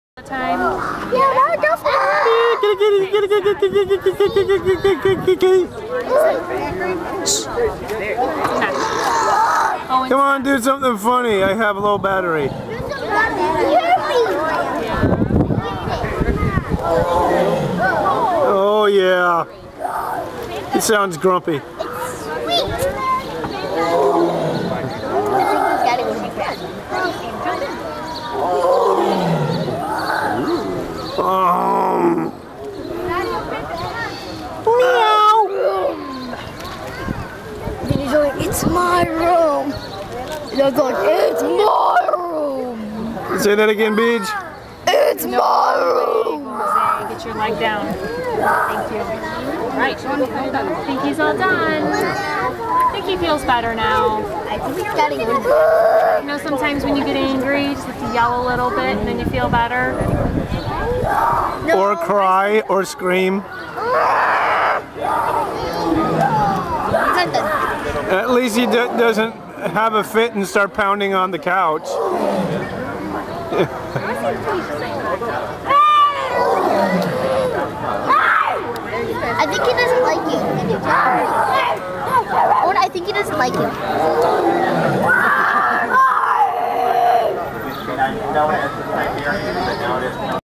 BIG tiger!
this is in Brookfield Zoo. first off this is the most physical and vocal i've EVER heard a tiger in any zoo that i have ever been to! sounds like he is grumpy or whatever! if anyone knows better what this guy is doing please let me know